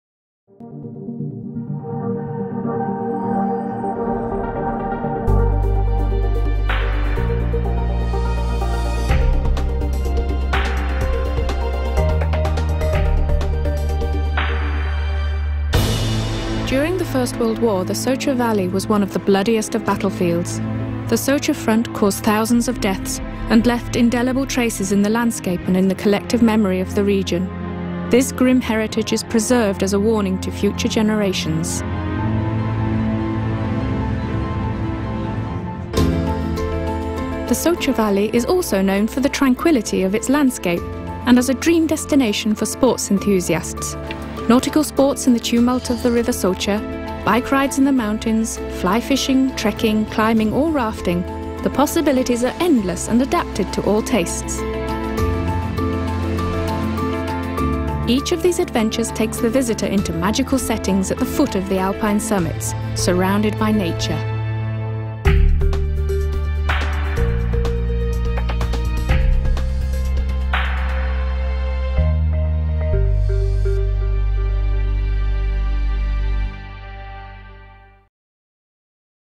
EDEN 2008 - The Soča Valley (Slovenia) short version
Situated in the heart of the Julian Alps, the area prides itself with one of Europe's oldest and Slovenia's only, national parks. Slovenia's first alpine botanical garden and snow-covered peaks with slopes provide the opportunity to enjoy a perfect view down to the sea